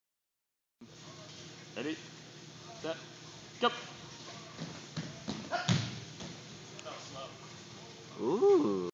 Port City Sports Performance-1.6 sec triple box jump
MMA fighter Kyle Whittington performs triple reactive box jumps at the begining of his training session to get his cns fired up. 1.6 sec was the best time of the day